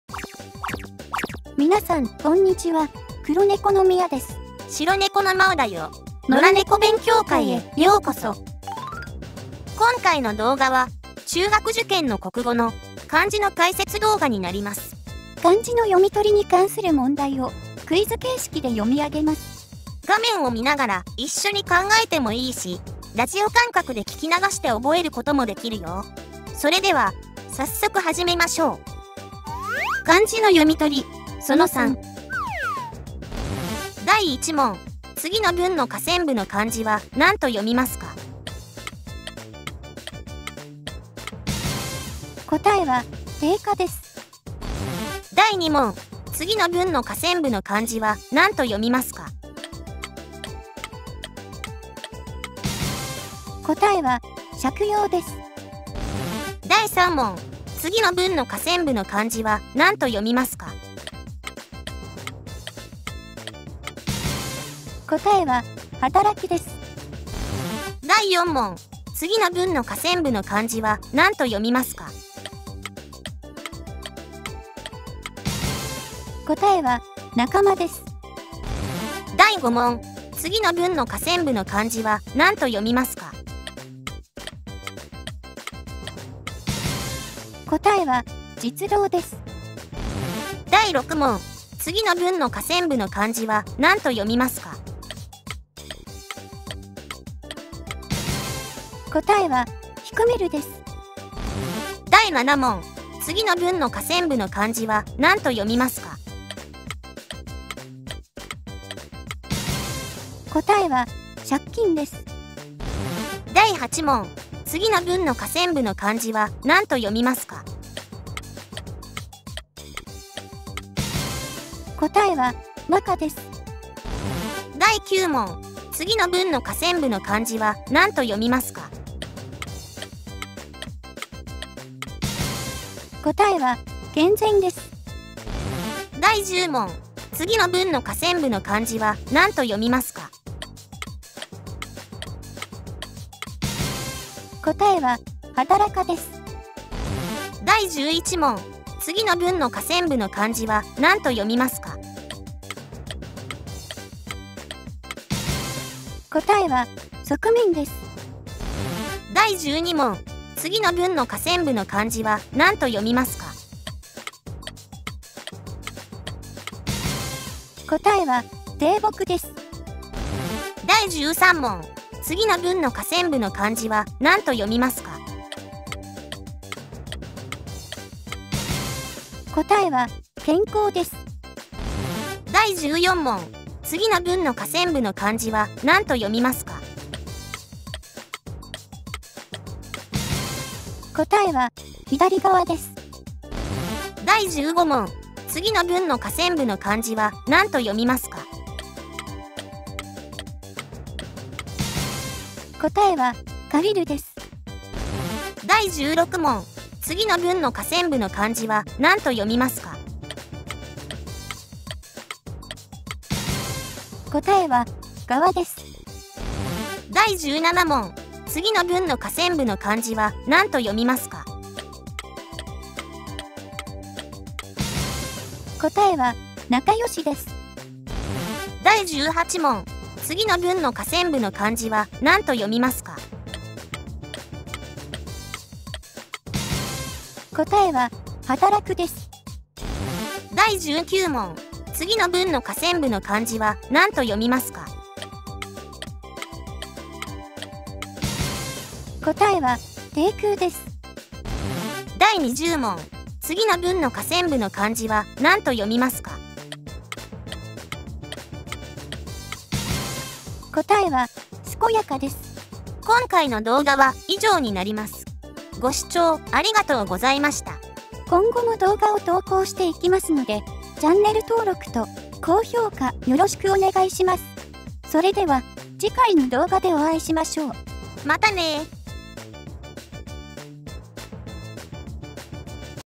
【漢字クイズ】小4漢字の読み取り一問一答・その3【中学受験/ゆっくり解説】
今回の動画は、中学受験の漢字の一問一答になります。
小学4年生の漢字の読み取りに関する問題をクイズ形式で読み上げます。
画面を見ながら一緒に考えてもいいし、ラジオ感覚で聞き流して覚えることもできます。
勉強が大好きなお饅頭猫たちと一緒に楽しく勉強しませんか？

【野良猫勉強会公式グッズ】
▶LINEスタンプ

【漢字のプレイリスト】

【動画目次】
00:00 オープニング
00:30 小4漢字の読み取り・その3
04:23 エンディング